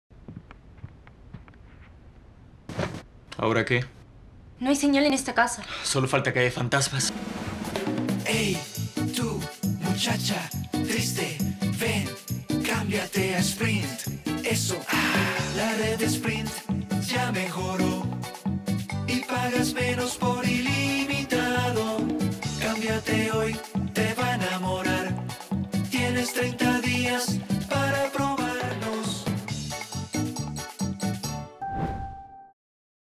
Sprint "Fantasmas" TV Commercial
Sprint keeps giving you more reasons to fall in love with their network. The original spot is voiced by the legendary Caribbean group and celebrates Sprint’s iconic plans and devices.